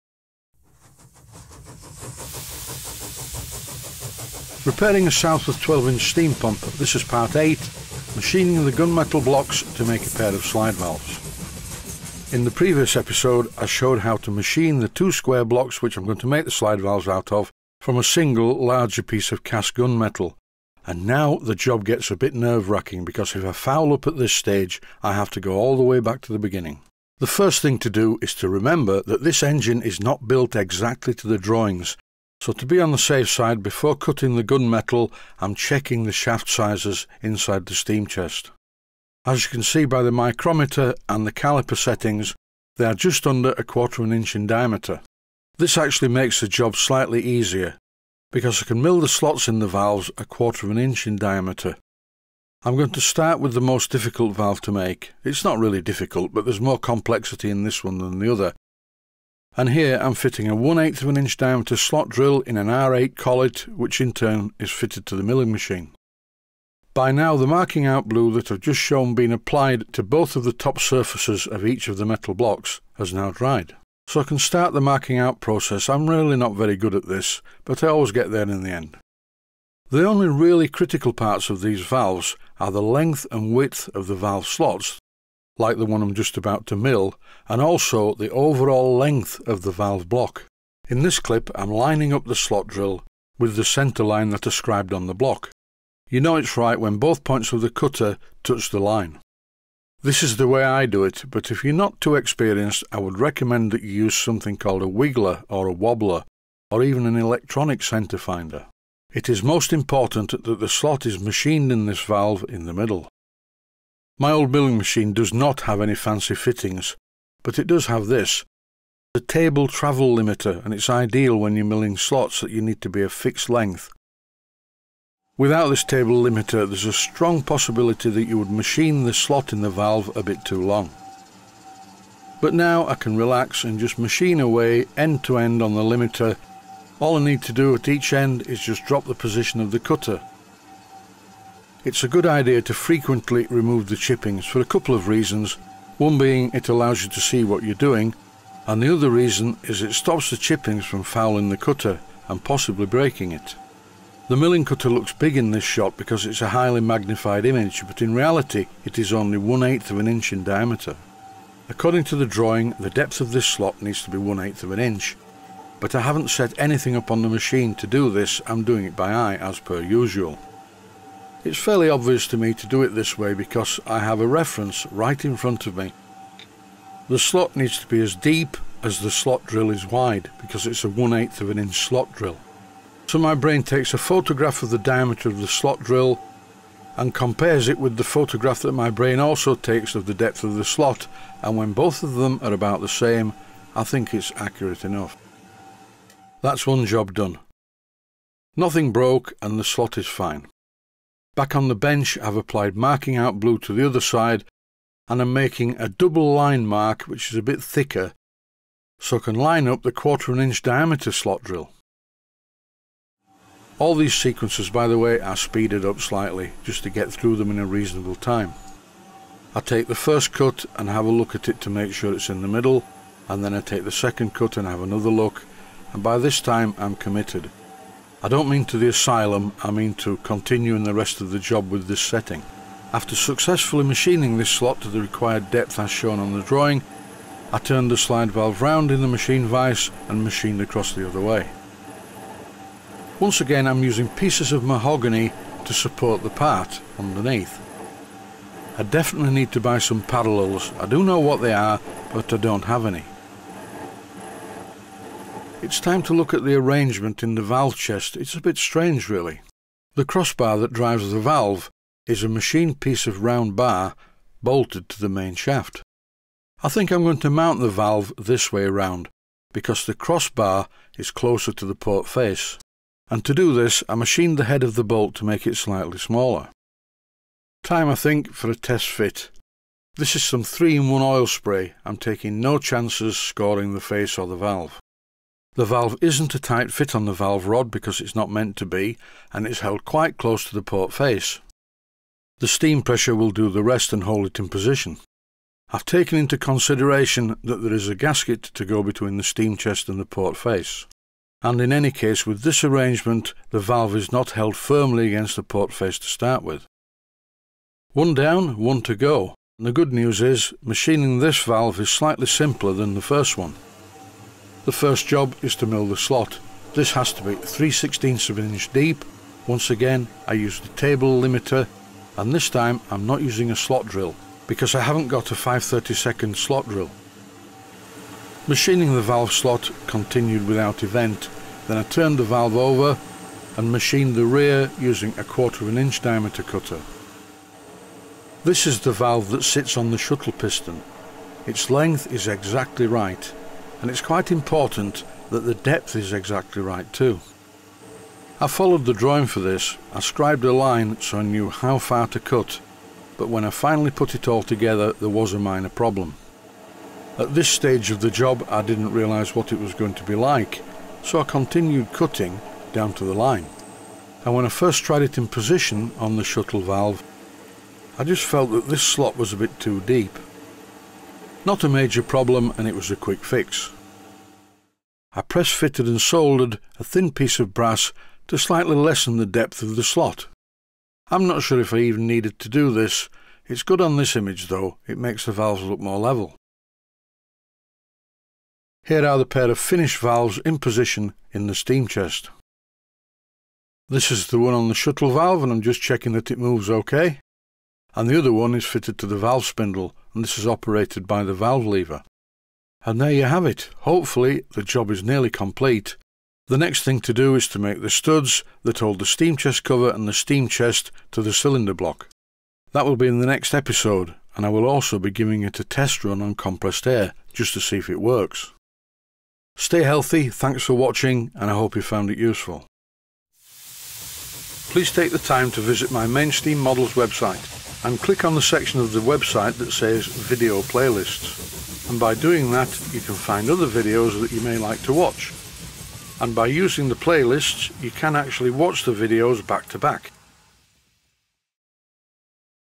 REPAIRING A SOUTHWORTH 12 INCH STEAM PUMP - PART #8
Repairing A Southworth 12 Inch Steam Pump - Part #8. Machining the Gunmetal Blocks to make a pair of Slide Valves.